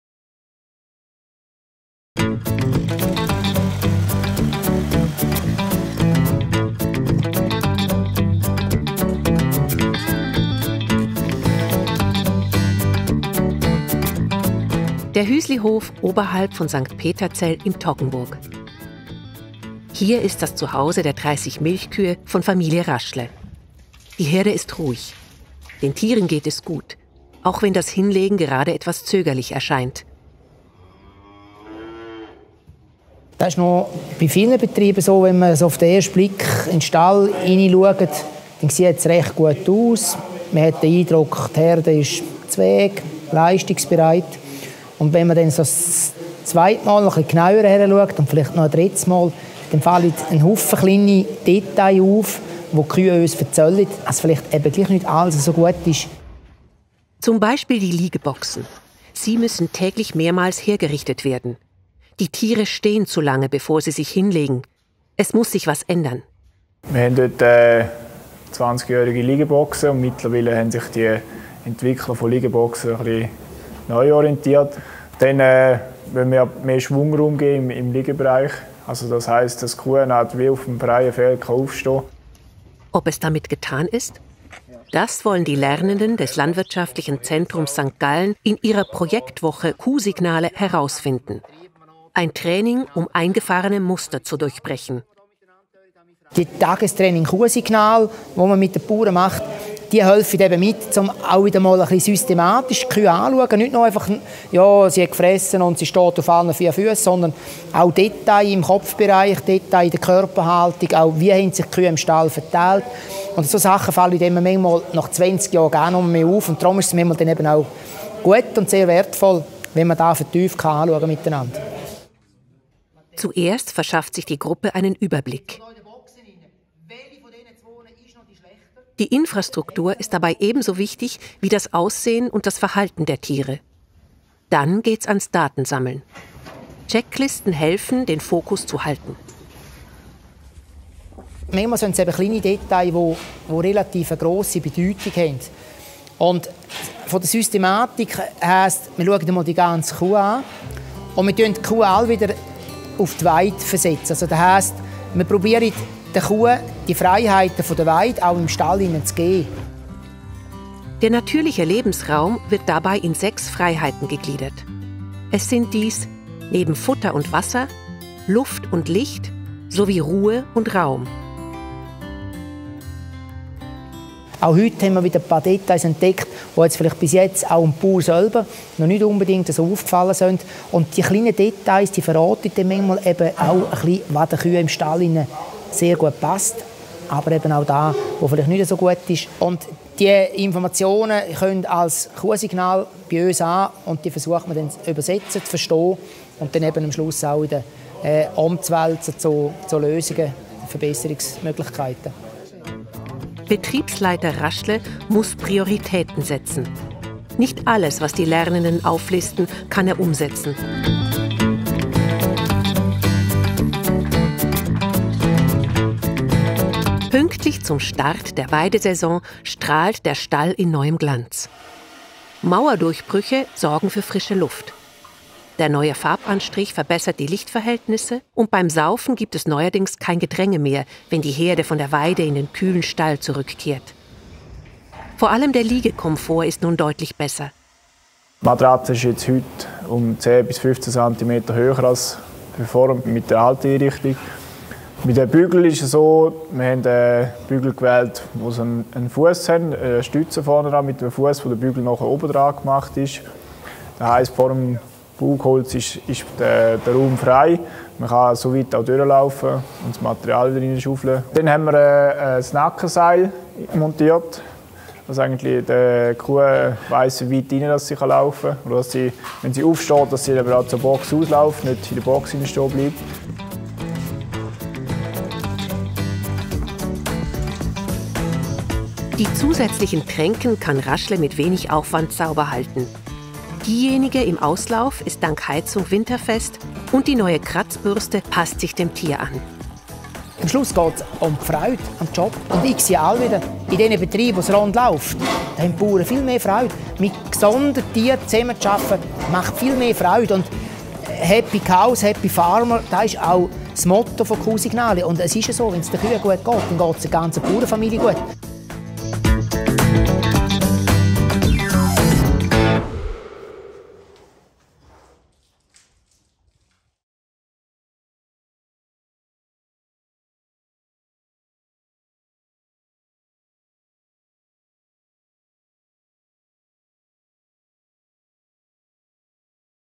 Fachvideo: Kuhsignale
Der Erfolg in der Milchwirtschaft hängt von zahlreichen Faktoren ab, die man als Betriebsleiterin oder Betriebsleiter nicht immer alle im Auge behalten kann. Mit der Zeit schleichen sich in der täglichen Arbeit zudem Gewohnheiten ein, welche die Leistungsbereitschaft der Tiere zusätzlich mindert. Das Fachvideo der UFA-Revue zeigt, wie man dank des Tagestrainings «Kuhsignale» am Verhalten und Aussehen der Kühe erkennt, was im Stall nicht passt.